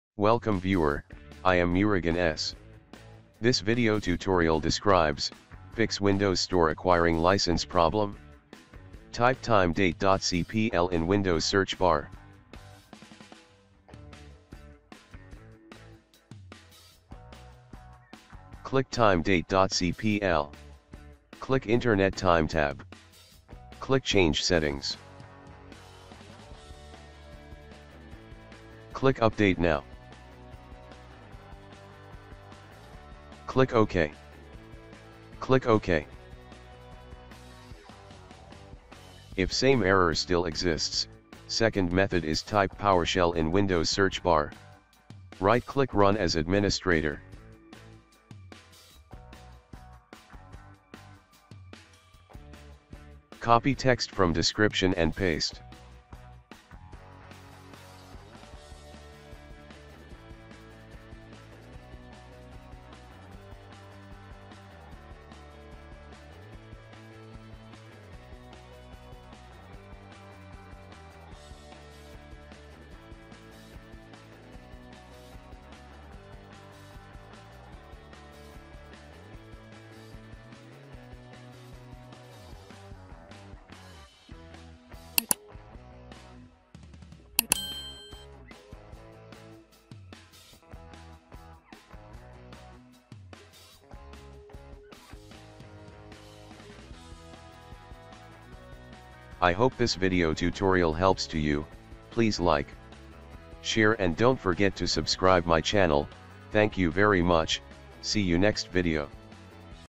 Fix Windows Store Acquiring License Problem
This Tutorial Helps to Fix Windows Store Acquiring License Problem